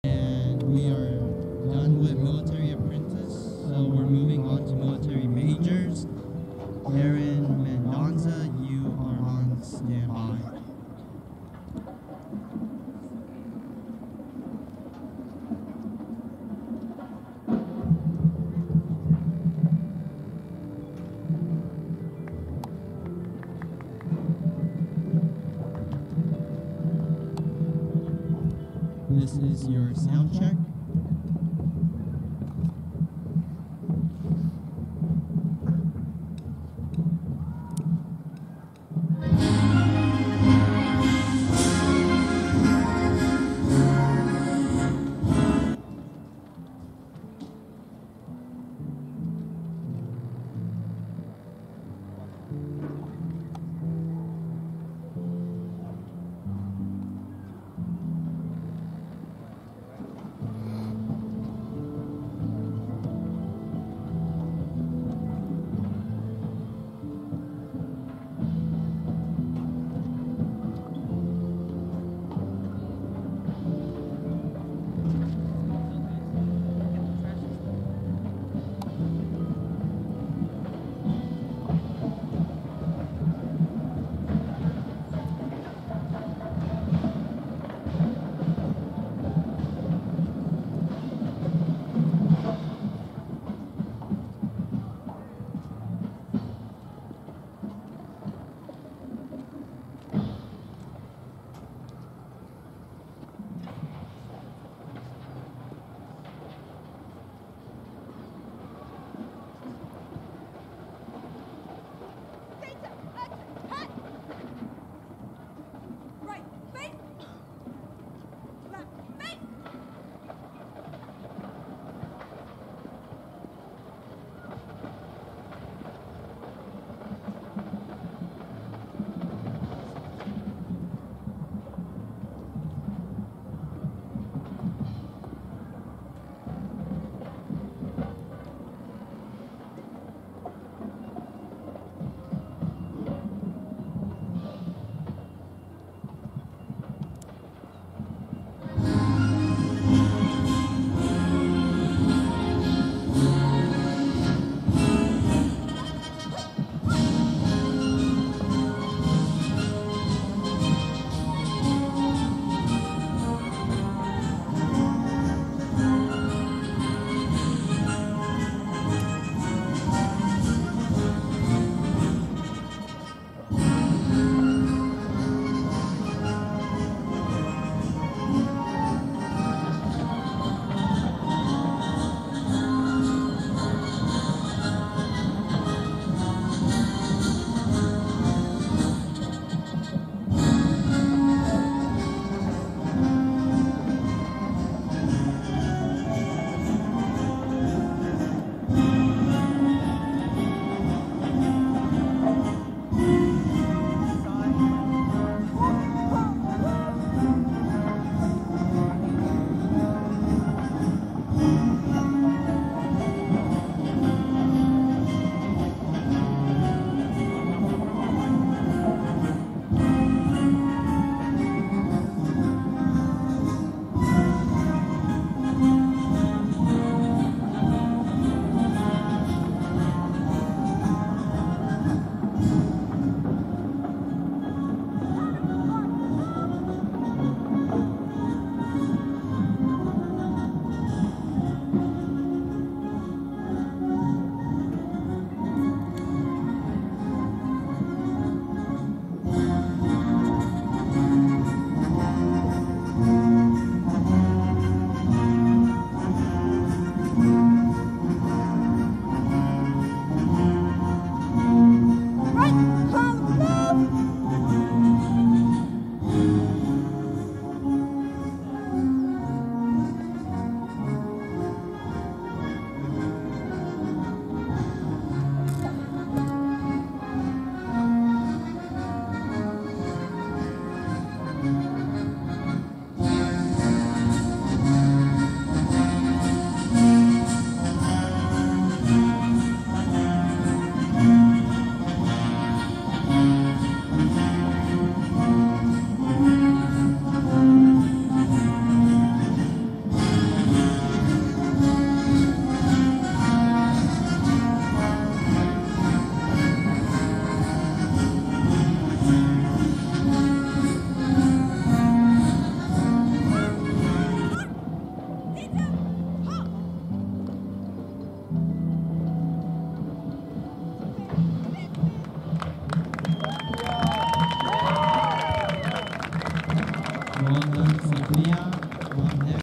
Sophia Moreno from Rodriguez HS on 3-21-15 at Elk Grove L-Pattern Competition
Sophia’s L-Pattern performance on 3-21-15 at Elk Grove HS, 9th Annual Herd Winter Review.   Sophia competed in HS Mace Apprentice.